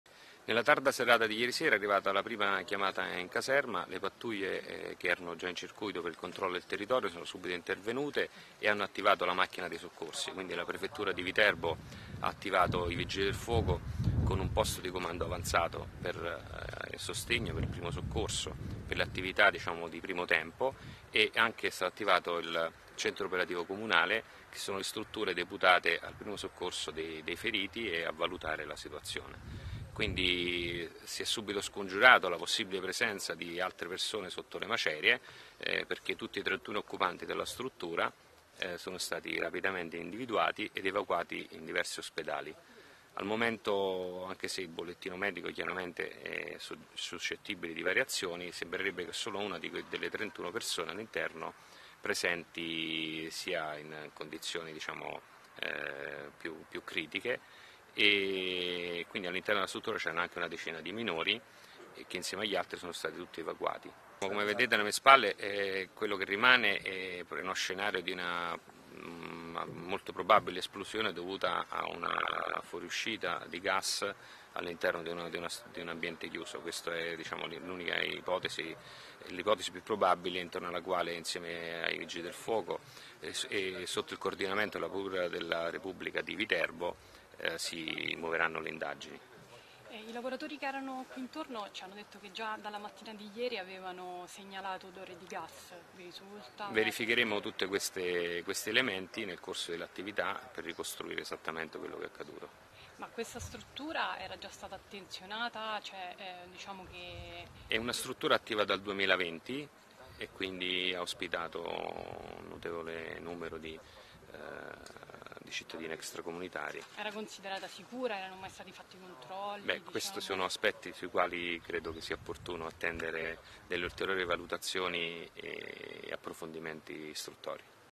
EsplosioneViterbo in centro accoglienza, Carabinieri: «Probabile fuga gas, verificheremo sicurezza»
Il punto della situazione con il colonnello Massimo Friano, comandante provinciale dei Carabinieri di Viterbo